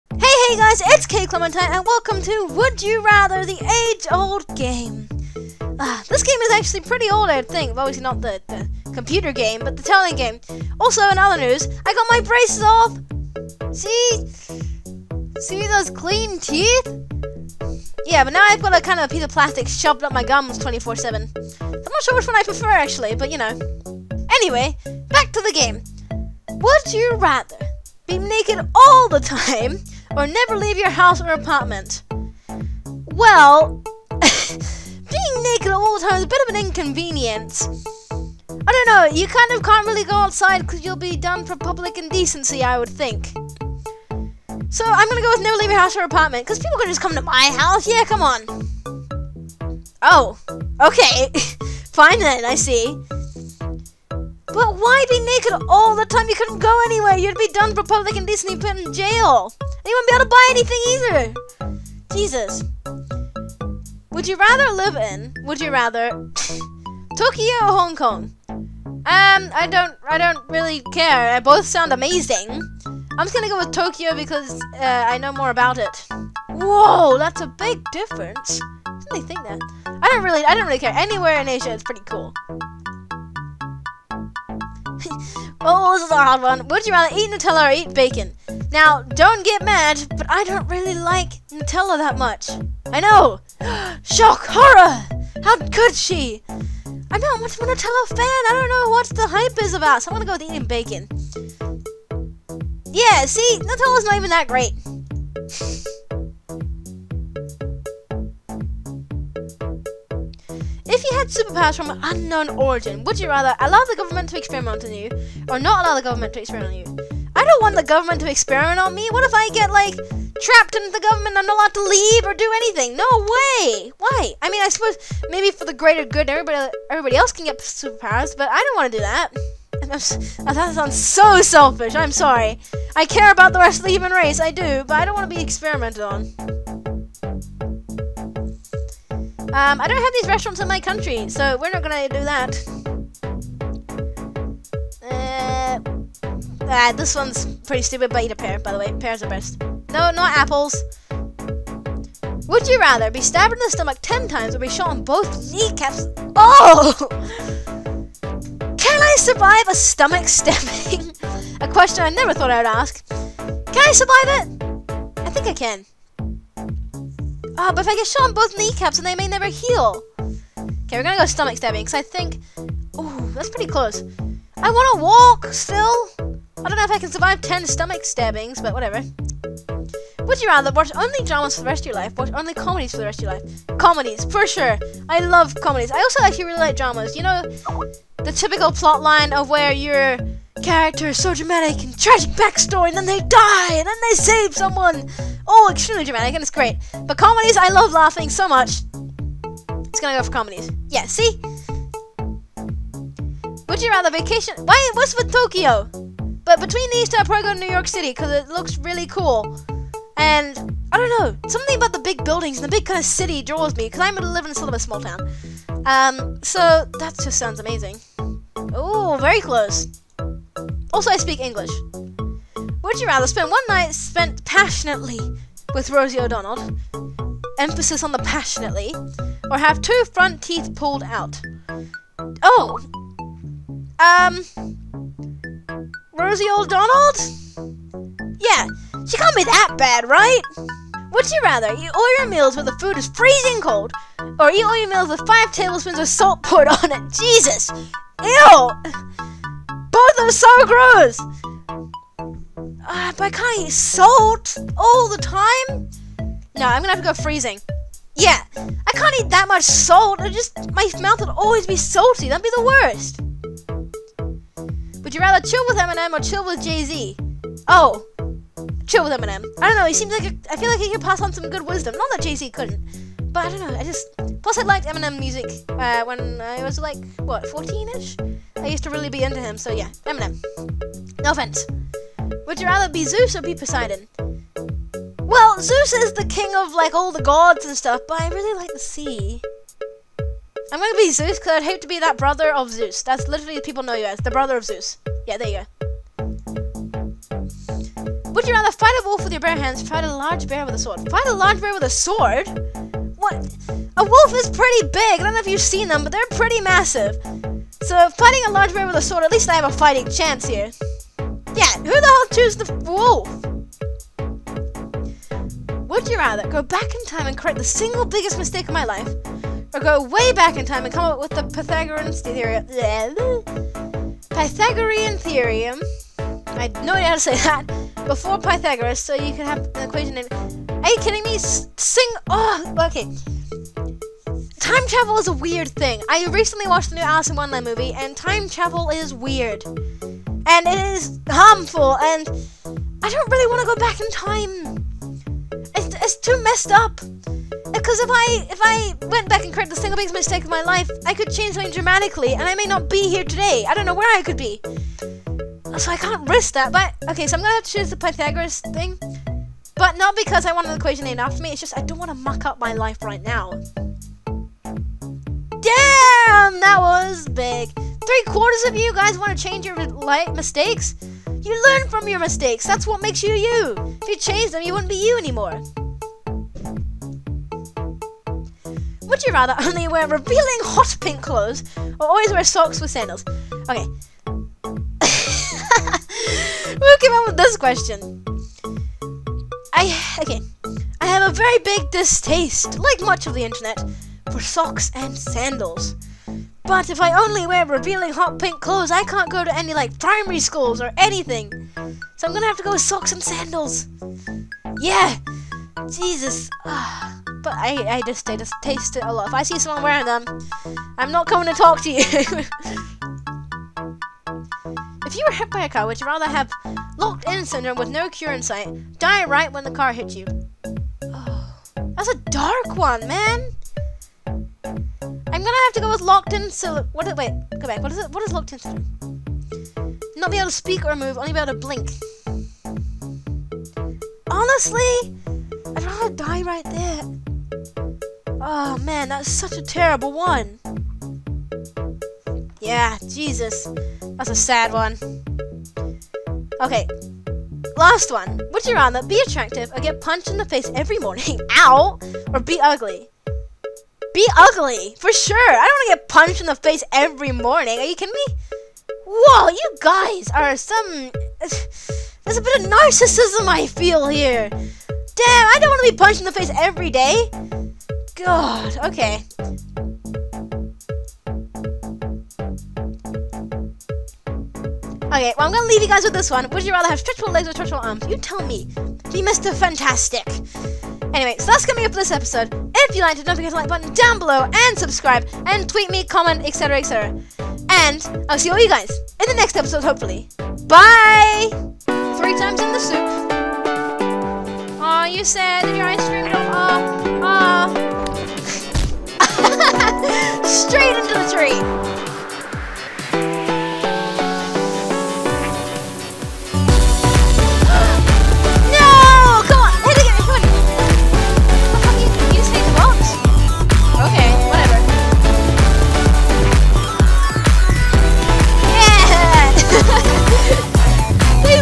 WEIGHING UP OPTIONS | Would You Rather #3
God there is some tough choices in this one. But also so very easy ones. Either way I usually pick he one that is again majority rule. Enjoy!